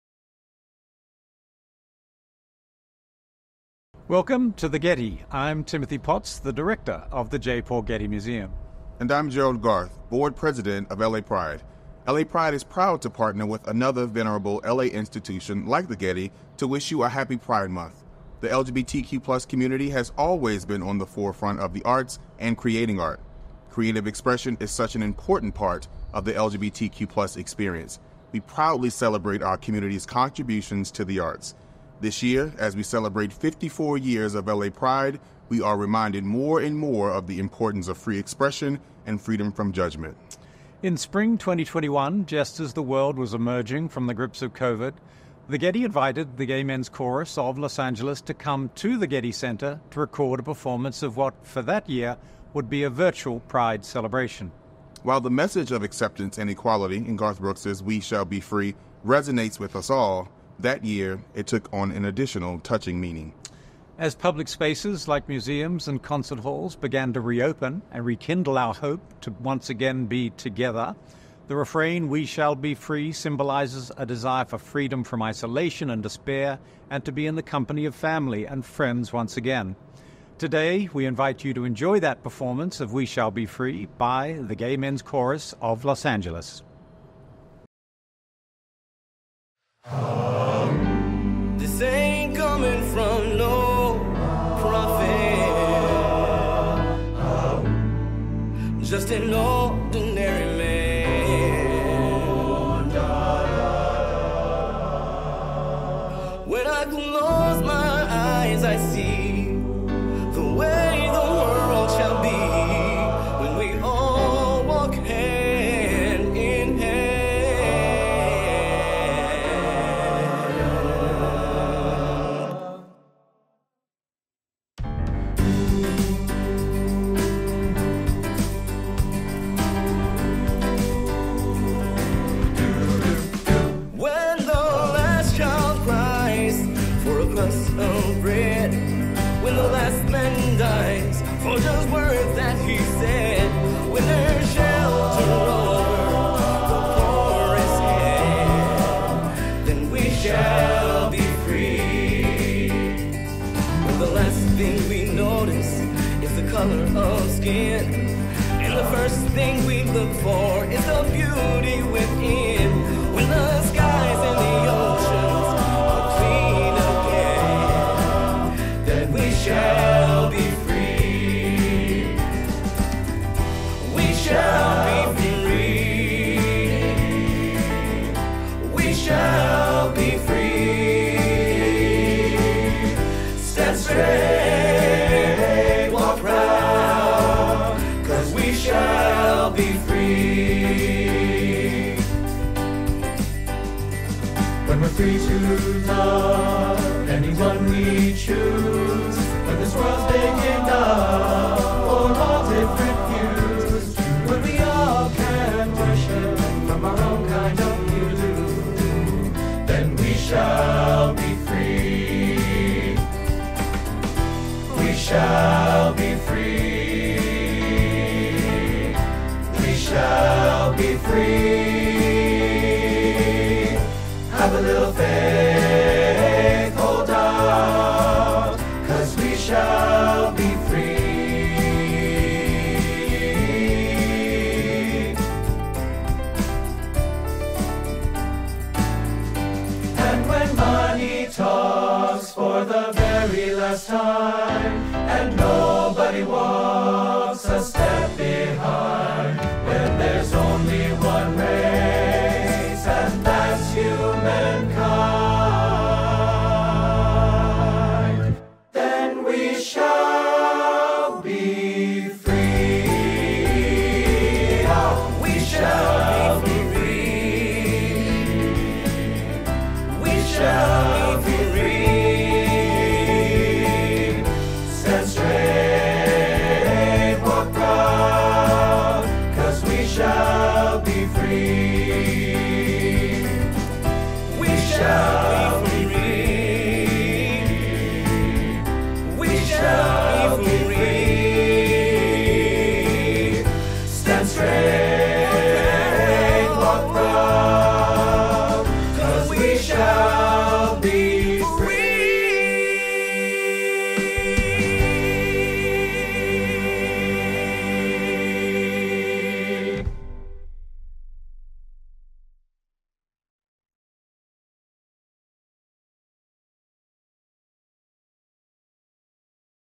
Gay Men’s Chorus of Los Angeles at the Getty Center
In celebration of Pride Month, we invite you to enjoy a recorded performance by the Gay Men’s Chorus of Los Angeles, filmed at the Getty Center.